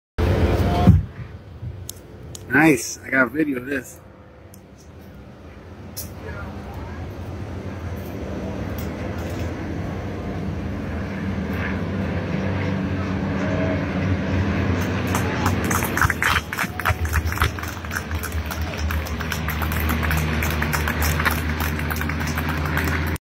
The Dreamland Squadron flyover during the Veterans Recognition Ceremony at Memorial Day RiverFest
This is a video of this year's flyover by City of Green Cove Springs Network Administrator Angel Alicea.